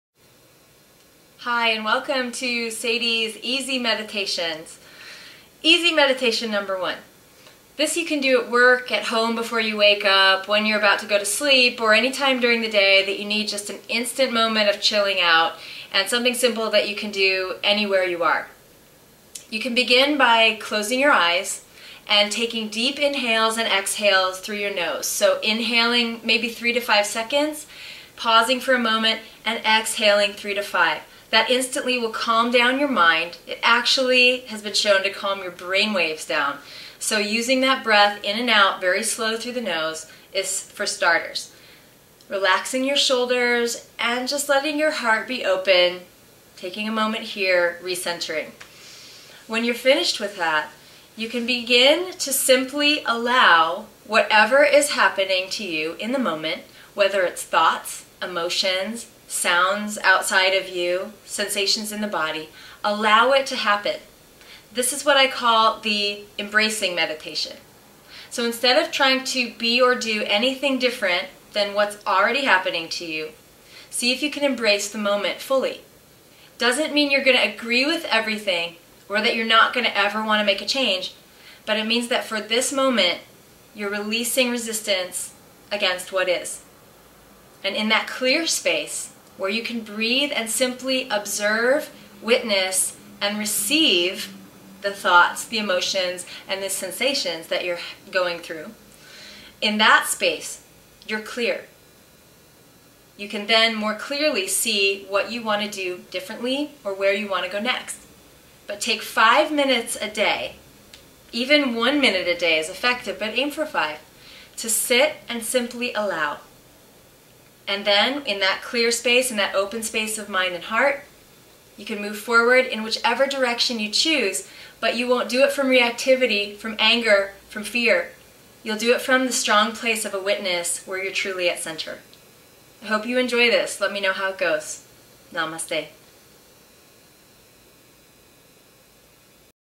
Yoga Stretch - SADIE'S EASY MEDITATION
Yoga Stretch - A SIMPLE DAILY MEDITATION TO HELP EMBRACE THE MOMENT, DISSOLVE CHAOTIC EMOTIONS/THOUGHTS AND GET YOU BACK TO YOUR CALM CENTER, QUICK! With Sadie Nardini, Master yoga trainer, Director of East West Yoga in NYC, Author of The Road Trip Guide to the Soul.